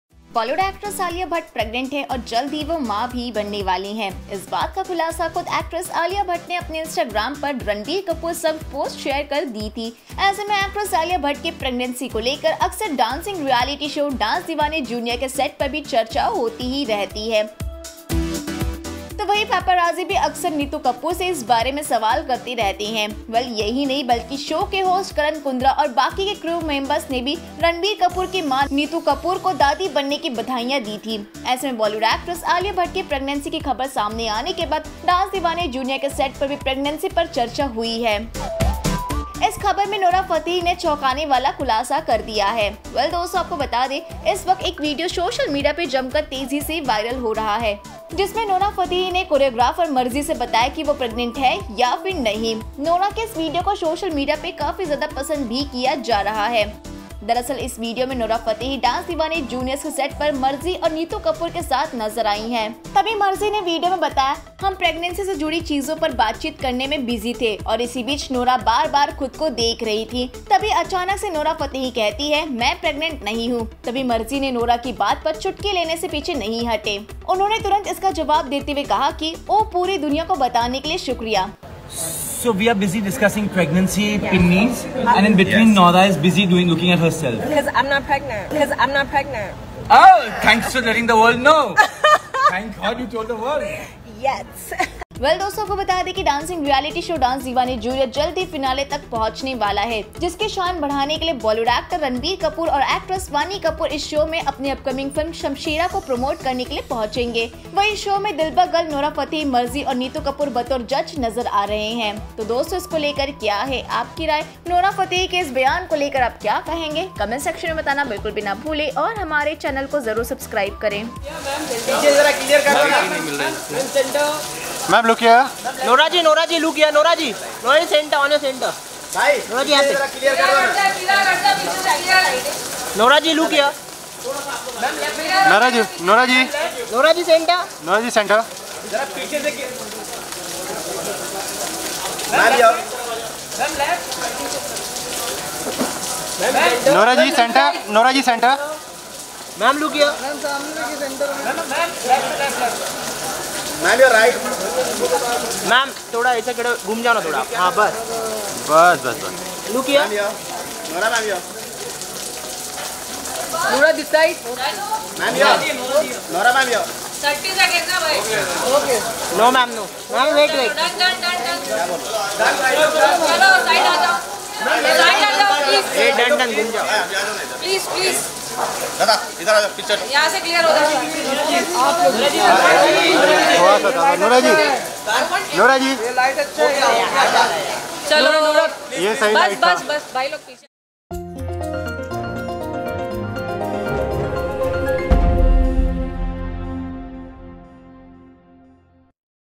Alia Bhatt के बाद क्या Nora Fatehi हैं PREGNANT ? एक्ट्रेस ने खुद किया खुलासा | Lehren Small Screen
बॉलीवुड की खूबसूरत अदाकारा आलिया भट्ट की प्रेग्नेंसी की खबर के बाद अब डांसर नोरा फतेही ने भी अपने प्रेगनेंसी को लेकर किया चौंकाने वाला खुलासा |